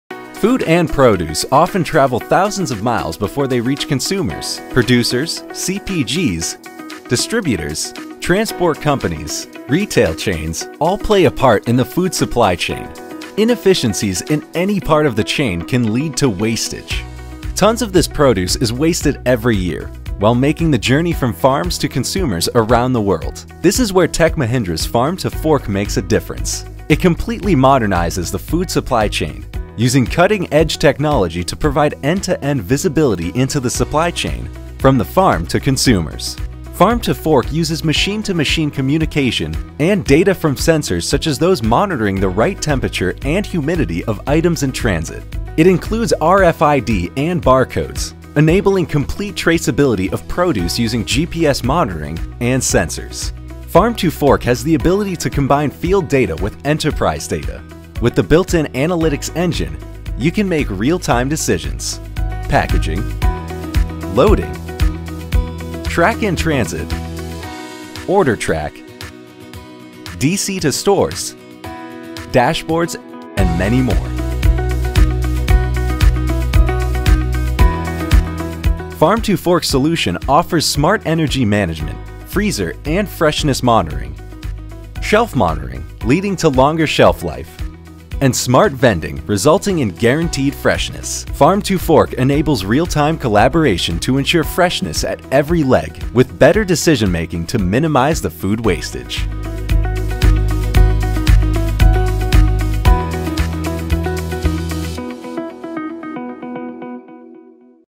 Farm to Fork
End to end visibility into the food supply chain for improved freshness of produce and food